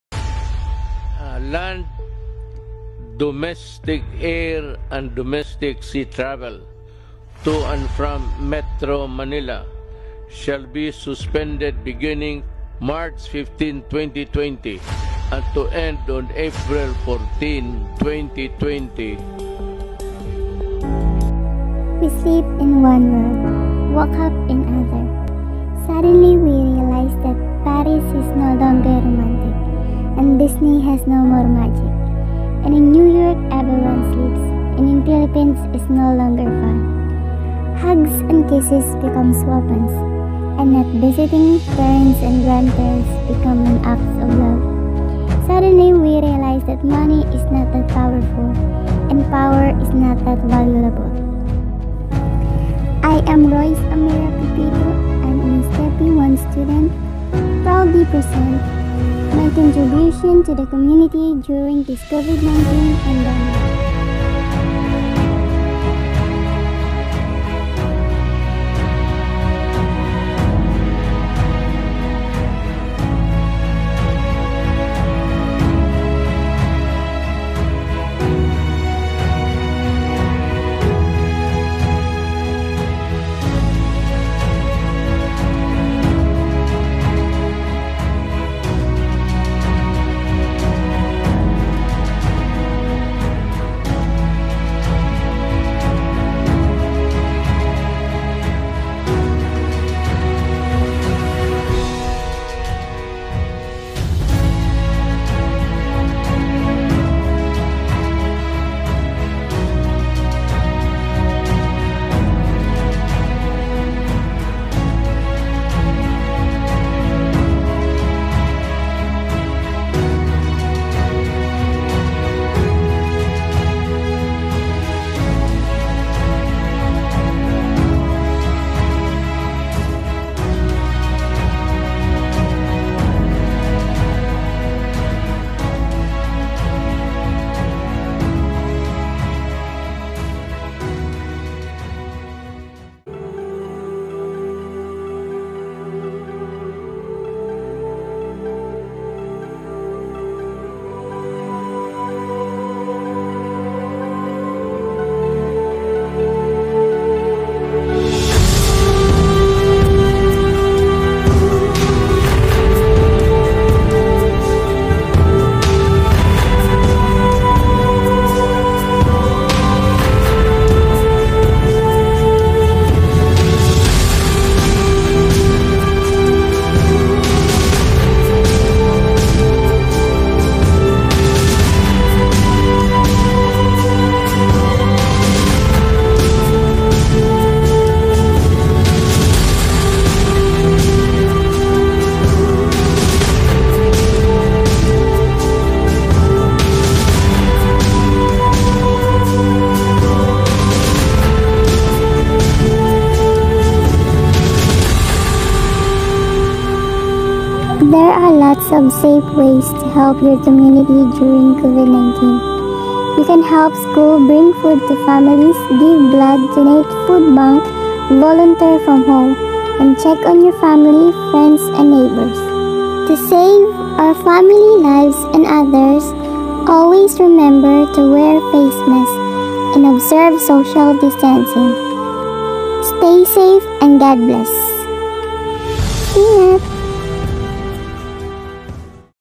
Save a day challenge ❗Not to brag but to inspire everyone 😇
Maliit na tulong sa kababayan natin save a day challenge 😇💪 sana masuportahan nyo pa kami para marami pa tayong matulongan na kababayan natin. kahit sa maliit na bagay basta taos pusong binibigay😇💓